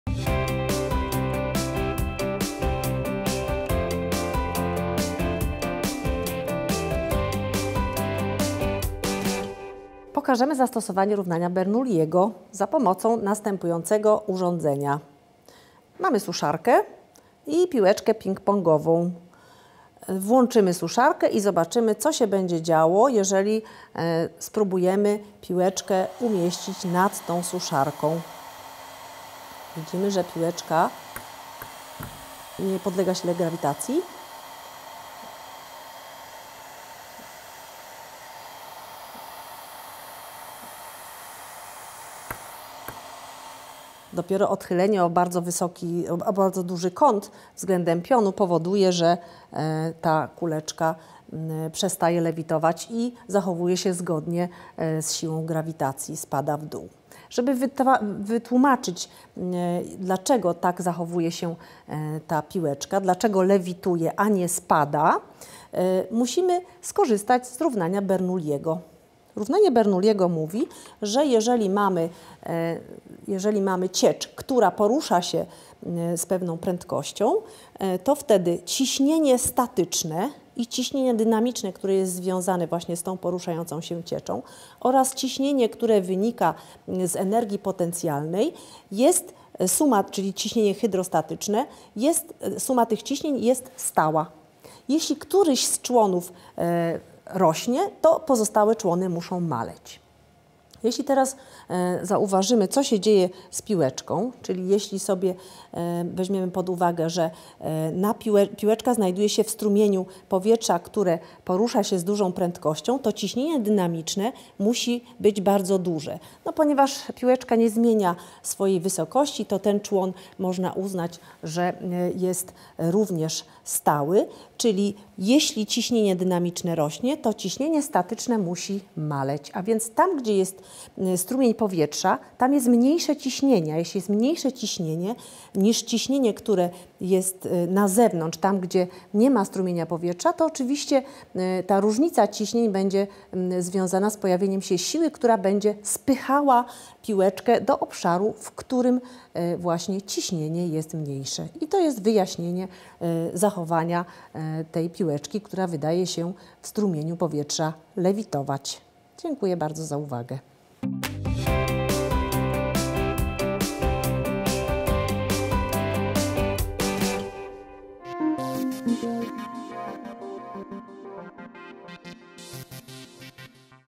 Fizyka I dośw. 10 - Lewitująca piłka
Fizyka I doświadczenie 10 - Równanie Bernoulliego - lewitująca piłka.